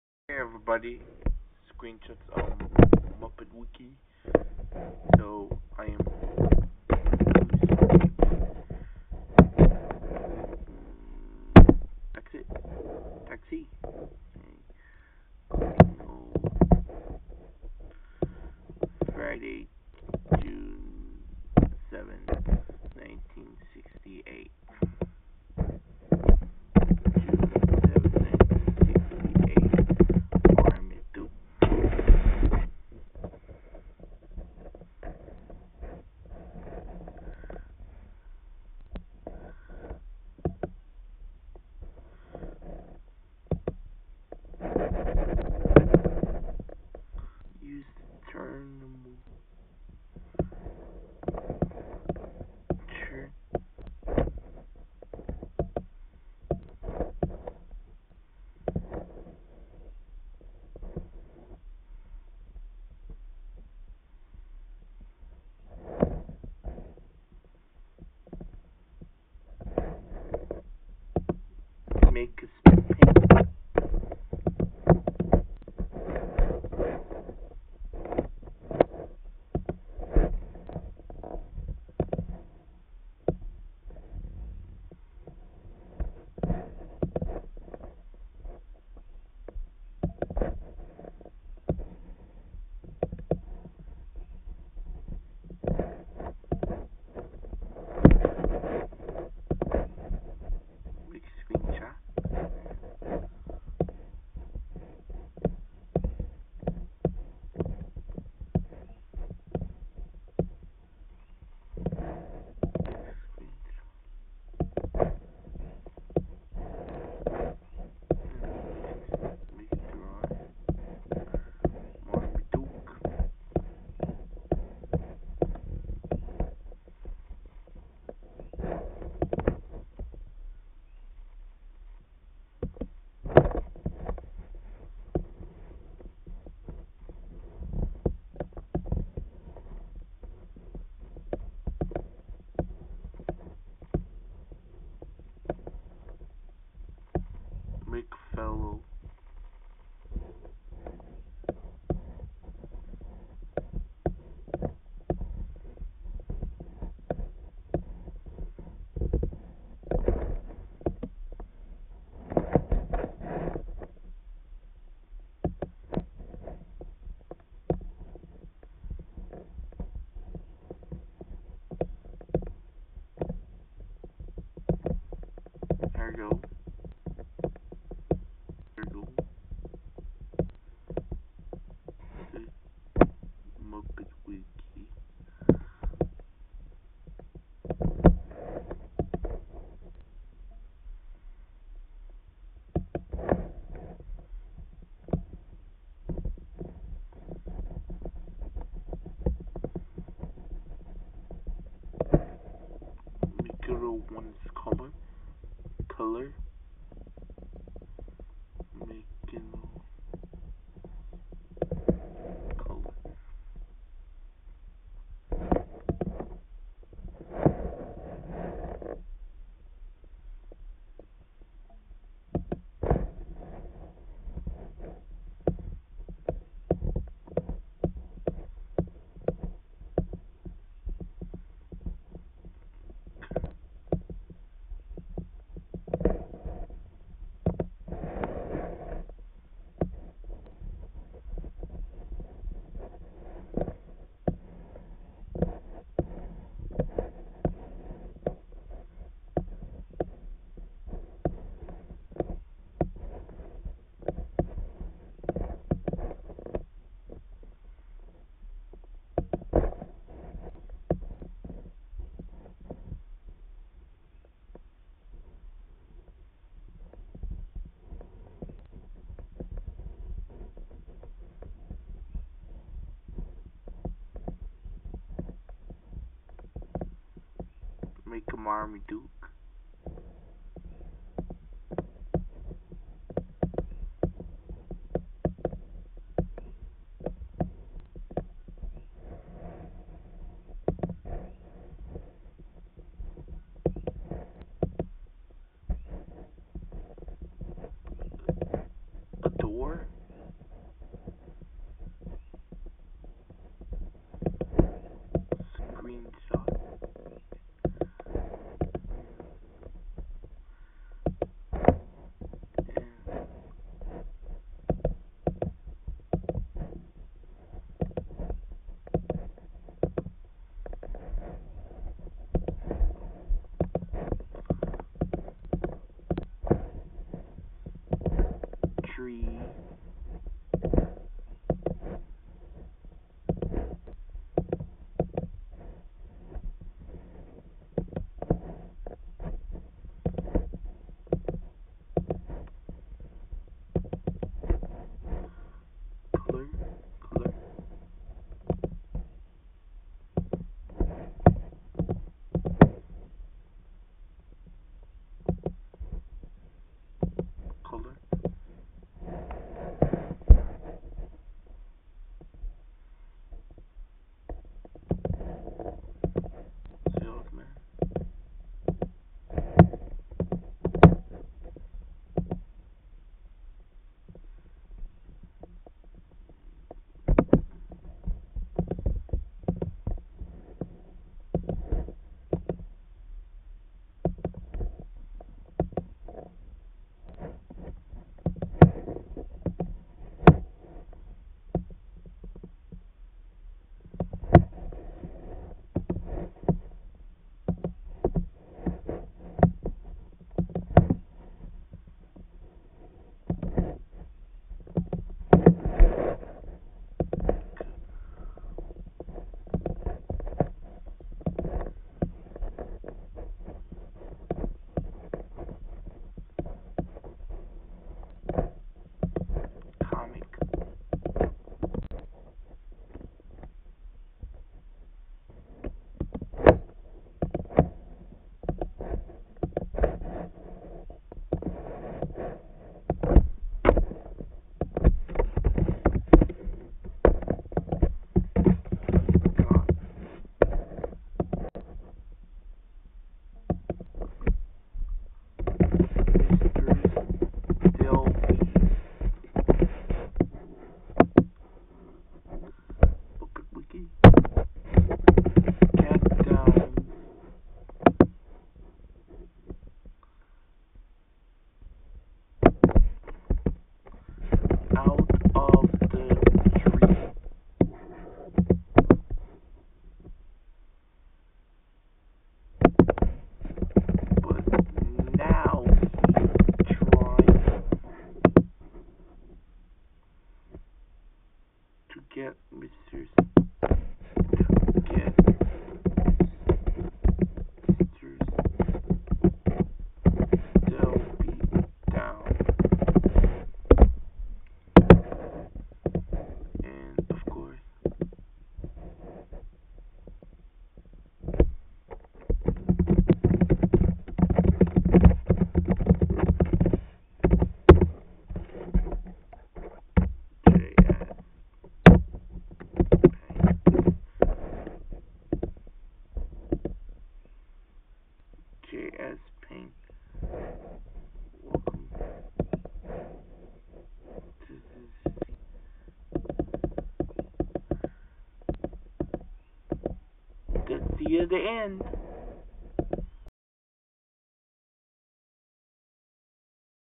How to Make Paint Marmaduke June 7 1968
JSPaint from June 7 1968
He got Mrs. Delby cat down out of the tree, but now he's trying to get Mrs. Delby down!
How to drawing Marmaduke June 7 1968 JSPaint PNG
Credit to anyone to this video.